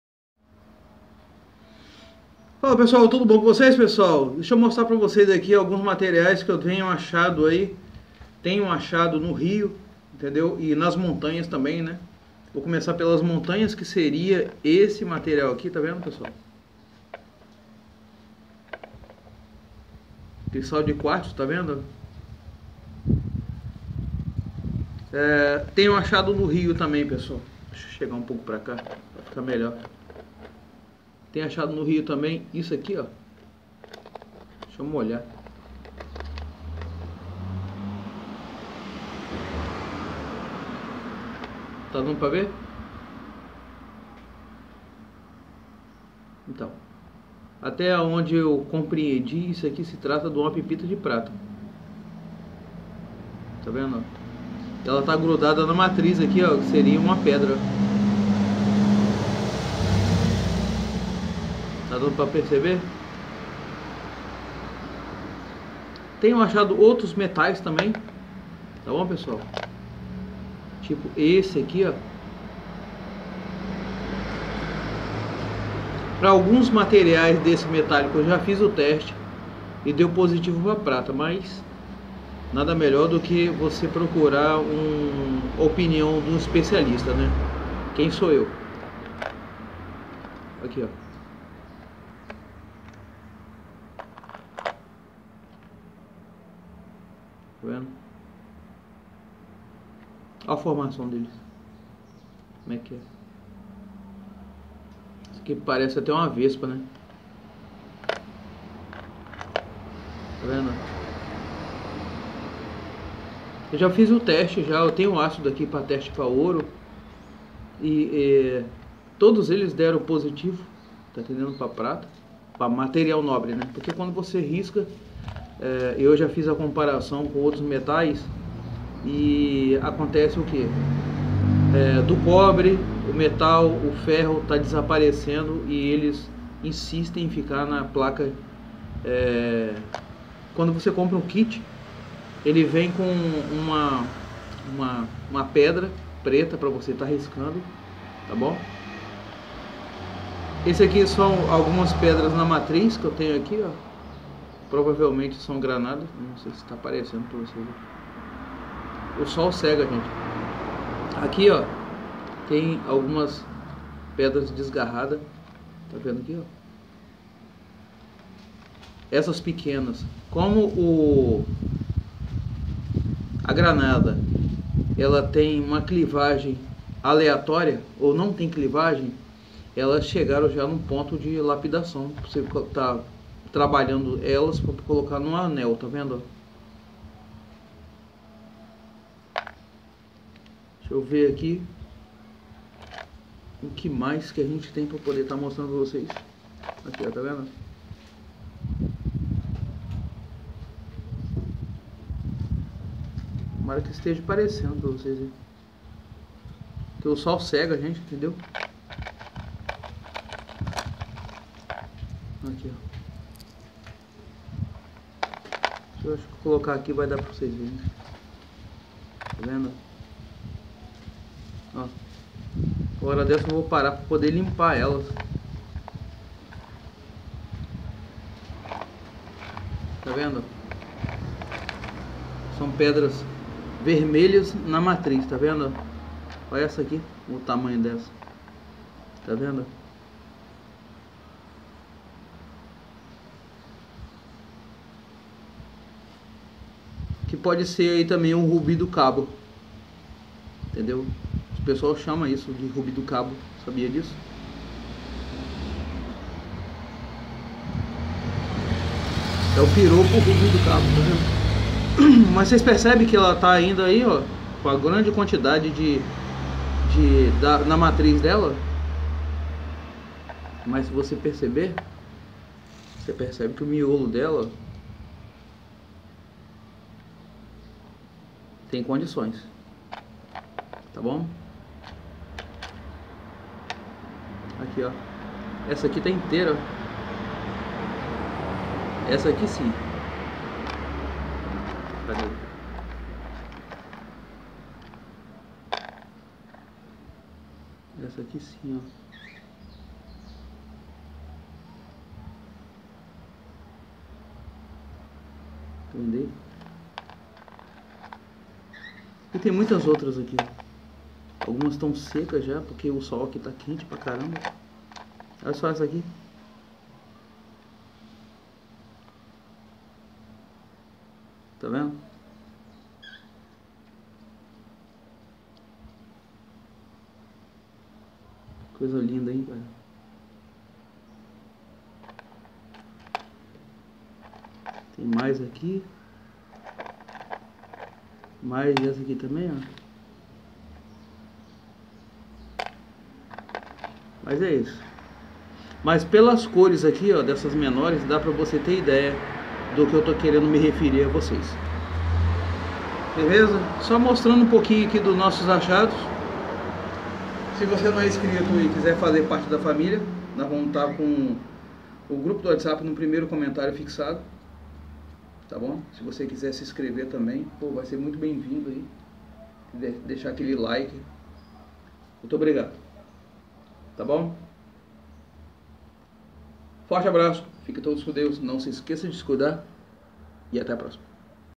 GARIMPO de METAIS e PEDRAS PRECIOSAS ACHADOS NOS RIOS ?? INSCREVA-SE !! PRECIOUS STONES
Olá pessoal nesse vídeo venho mostrar alguns achados de garimpo de metais e pedras preciosas.